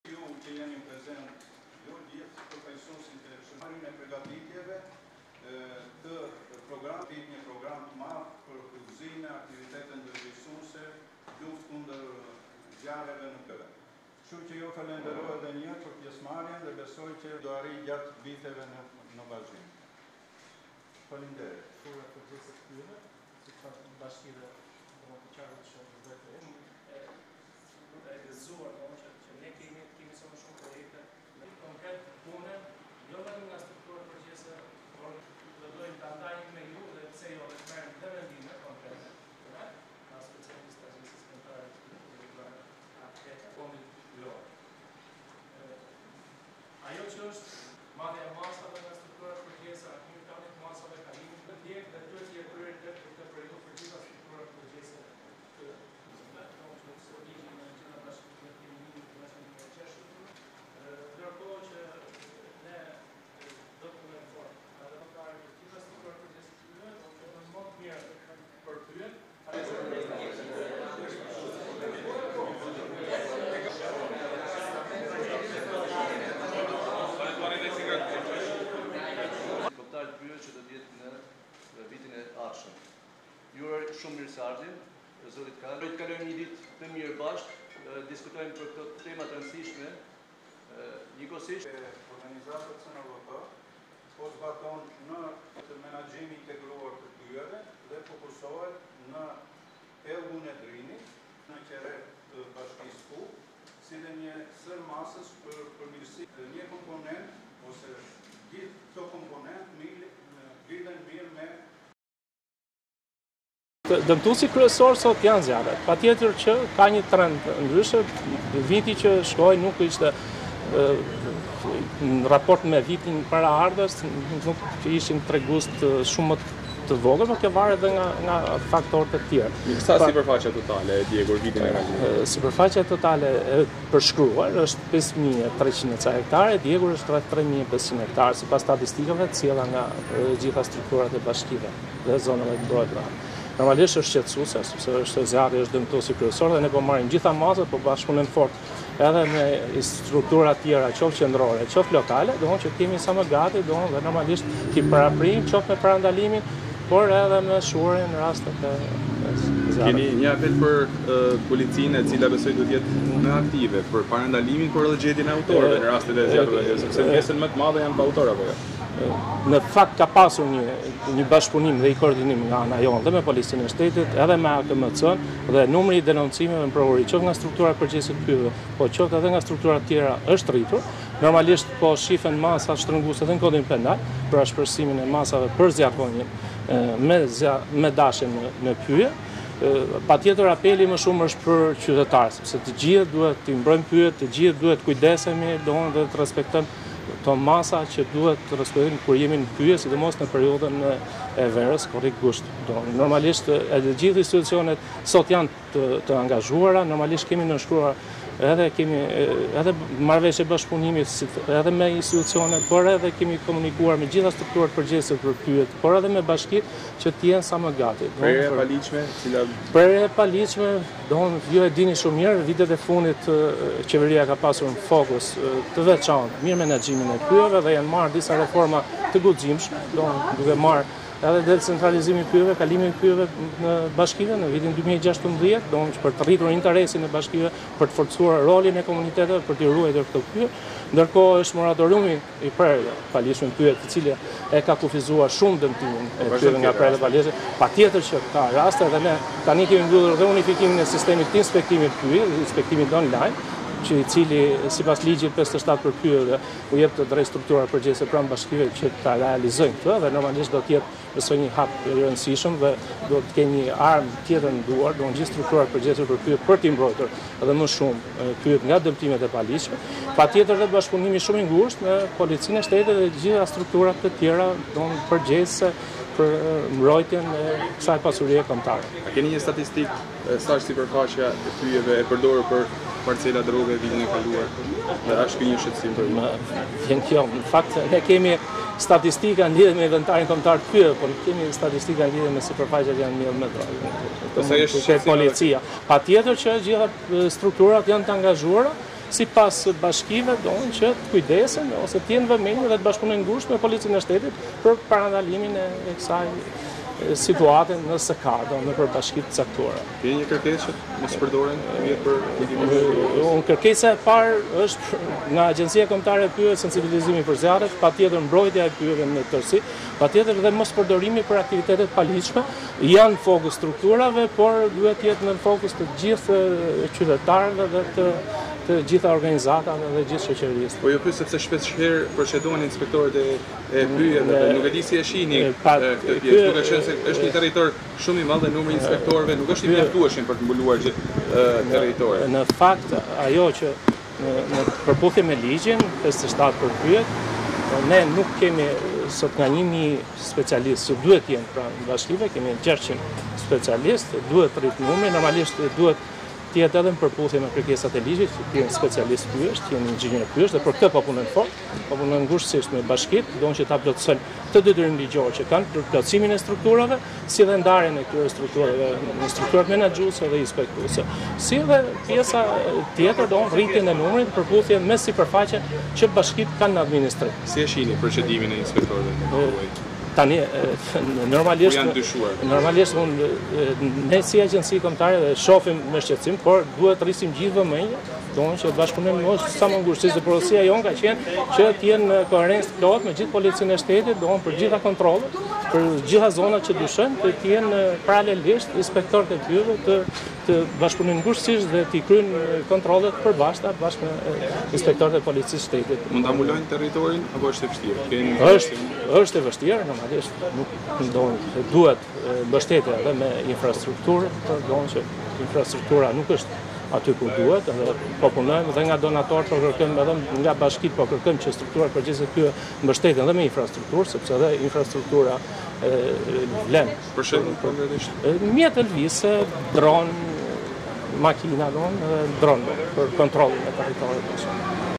Zjarret në Shkodër kanë “përvëluar” 3500 hektarë pyje, piromanët do dënohen
Nga Shkodra, drejtori i përgjithshëm i Agjencisë Kombëtare të Pyjeve, Artur Kala, theksoi se dëmtuesi kryesor i pyjeve sot janë zjarret, teksa në vitin e kaluar ka pasur rreth 3500 hektarë pyje të djegur. Ai tha se po shihen masa shtrënguese në kodin penal për ashpërsimin e masave për zjarrvëniet me dashje…Shqetësim mbetet edhe mbjellja e kanabisit në pyje, ku Kala thotë se duhet bashkëpunim i ngushtë me policinë…Gjatë takimit të zhvilluar për menaxhimin e qëndrueshëm të pyjeve në Qarkun e Shkodrës, Kala i bëri apel qytetarëve që të mbrojnë pyjet.